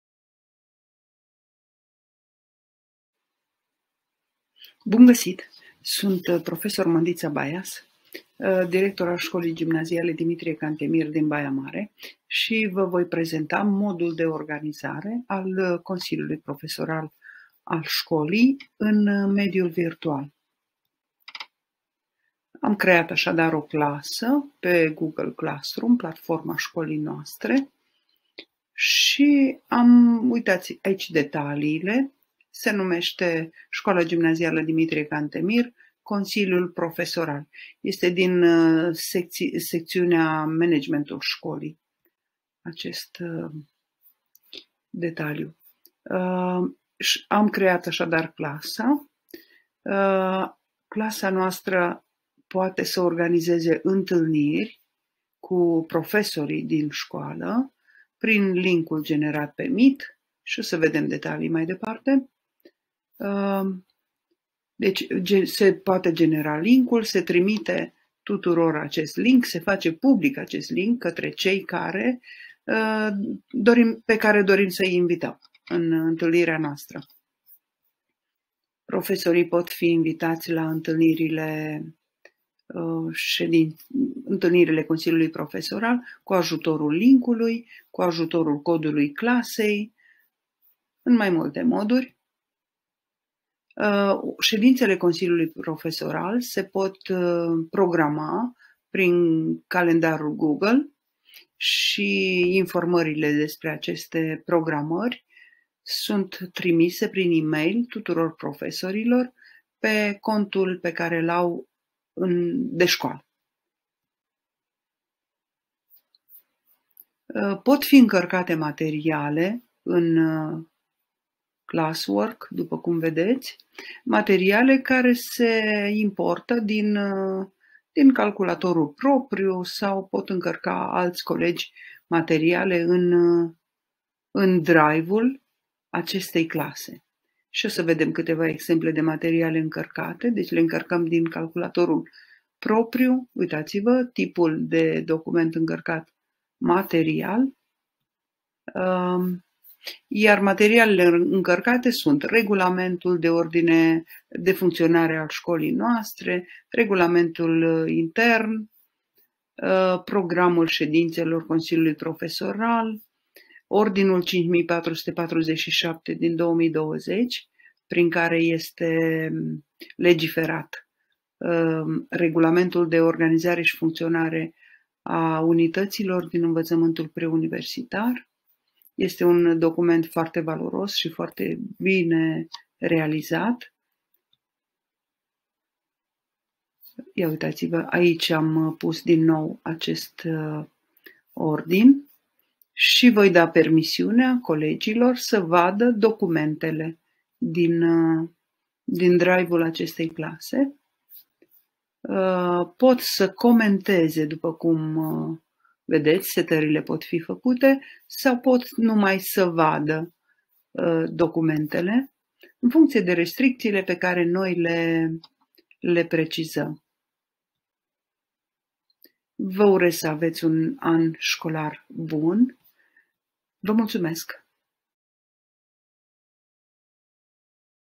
Consiliul profesoral al școlii
Această resursă educațională deschisă de tip video este un ghid care vine în sprijinul directorilor de școală în vederea folosirii materialului ca sugestie de management al diferitelor procese din școală.
Vă prezint un mod de organizare al activităților și materialelor Consiliului profesoral.
Este un material realizat cu ajutorul aplicatiei Canva